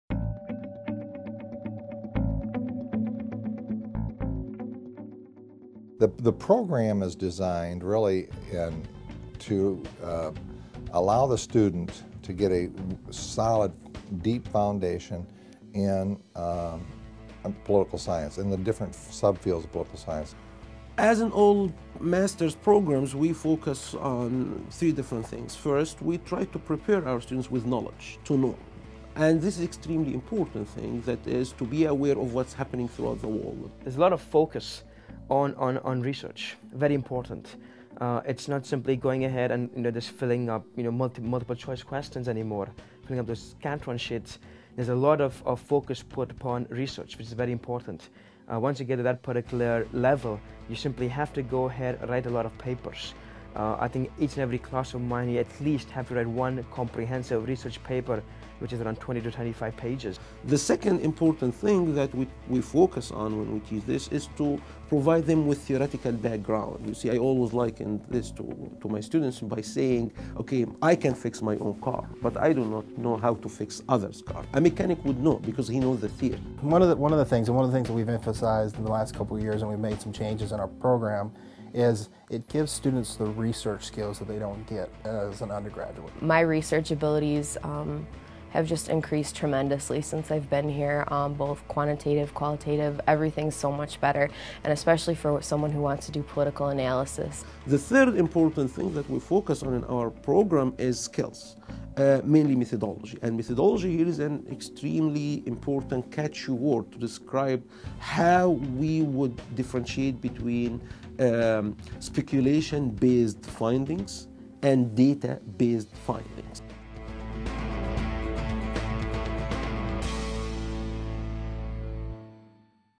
Student Life at Central Michigan University
Highlights what it's like to be a student living in Mount Pleasant, Michigan.